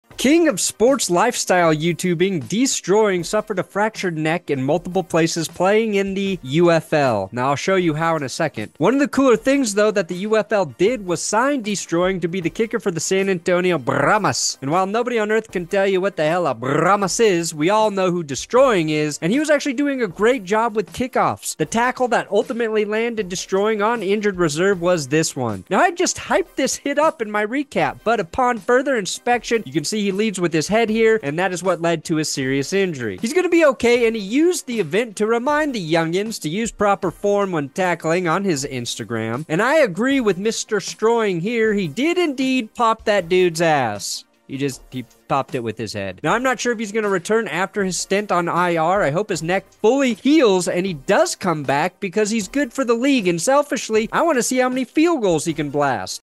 Deestroying Fractures Neck in UFL Game
San Antonio Brahmas kicker & Youtuber deestroying fractured his neck in multiple spots making a tackle in a week 2 ufl game.